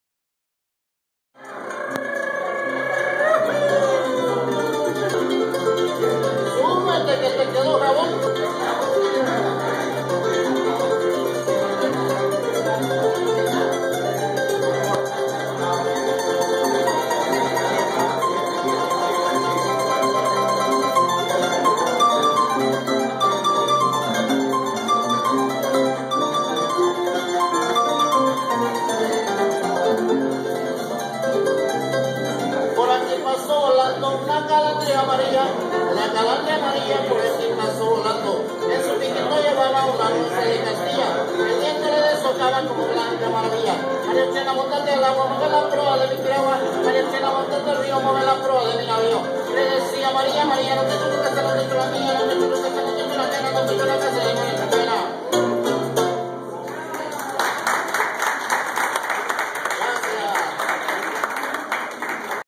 Valle de Guadalupe, música en la mesa
Música en la mesa, Mustafa's Restaurant, Valle de Guadalupe, Baja California, Mexico, Dec. 28, 2011